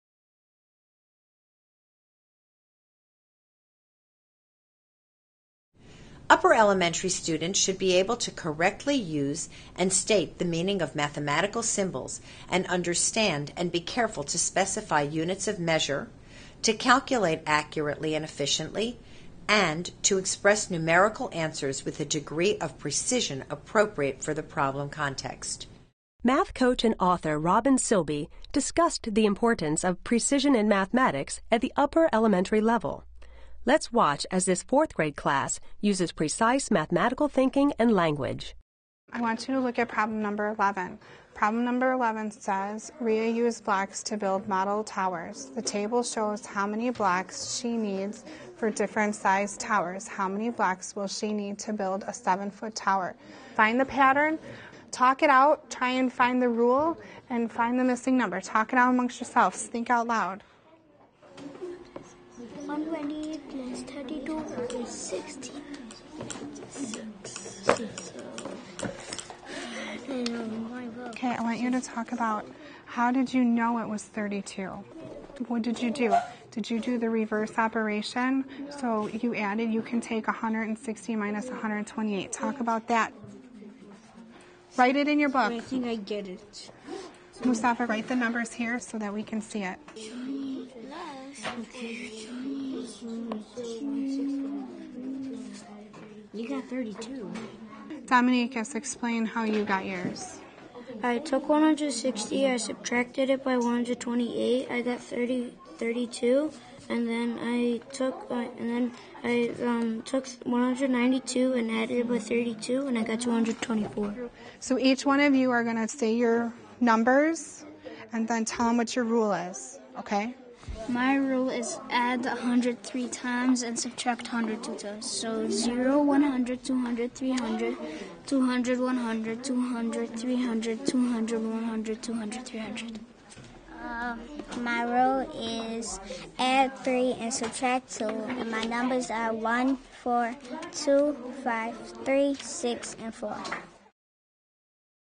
How to Teach Mathematical Precision
In this short video, watch as a middle school class applies Mathematical Practice 6 when the students and teacher use precise mathematical vocabulary to discuss a real-world problem.